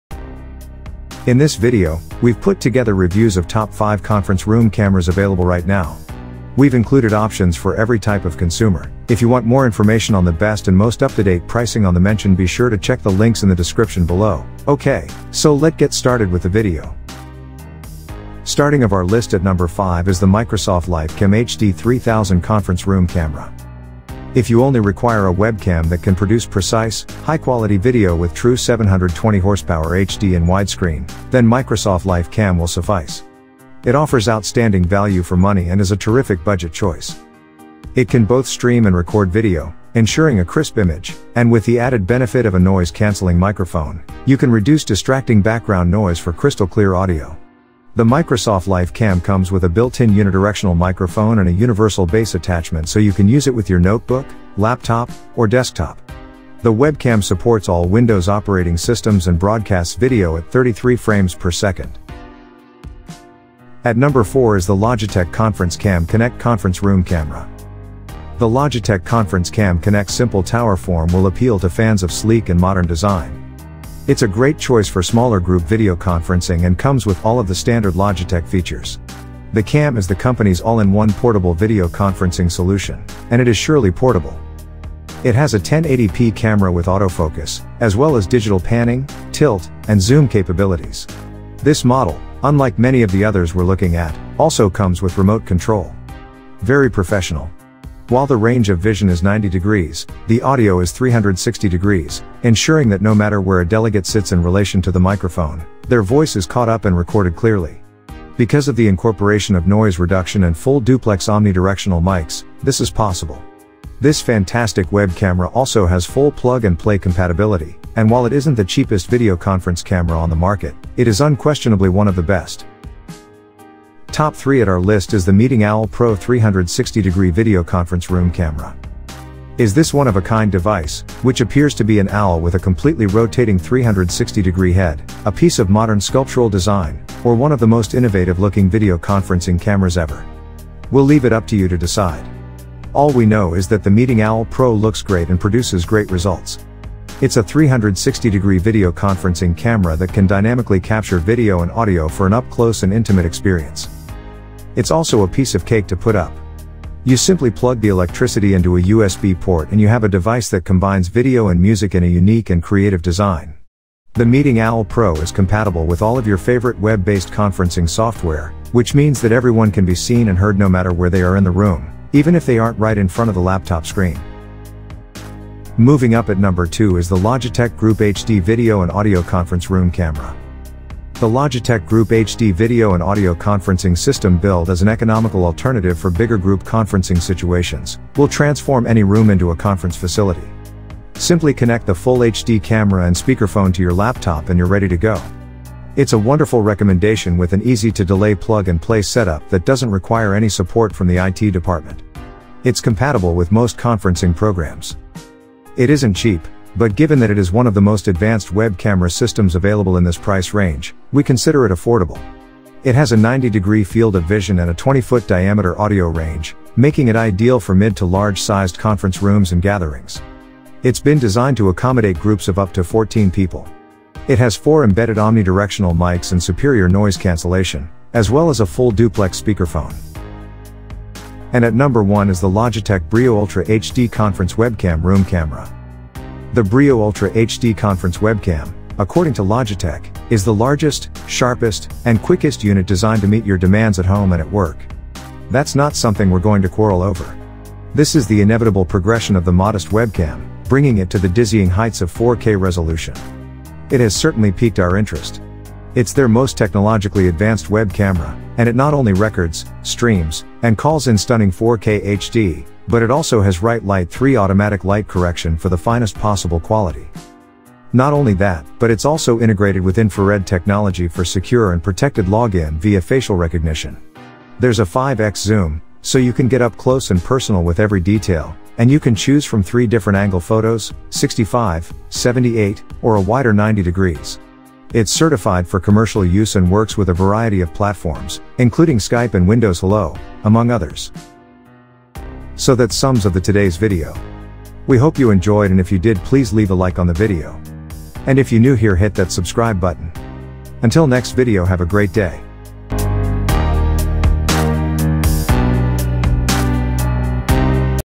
Zoom In on Excellence: 5 Best Conference Room Cameras for 2023
1️⃣  NDI 4K Video Conference Camera.

2️⃣  4K 10/20X Conference PTZ Camera.

3️⃣  Conference Camera PTZ Video HD USB 10X+16X Digital Zoom Camera.

4️⃣  SMTAV Conference Camera 1080P.

5️⃣  Conference Camera PTZ Video HD USB HD.

00:00​ Intro: This is a review of best Conference Room Cameras :
00:22 5⃣  Microsoft LifeCam HD-3000 Conference Room Camera
01:08 4⃣  Logitech ConferenceCam Connect Conference Room Camera
02:08 3⃣  Meeting Owl Pro 360 Degree Video Conference Room Camera
02:59 2⃣  Logitech Group HD Video And Audio Conference Room Camera
03:55 1⃣  Logitech Brio Ultra HD Conference Webcam Room Camera

Electronic Zone always tries to pick and make top trending and popular product lists for you.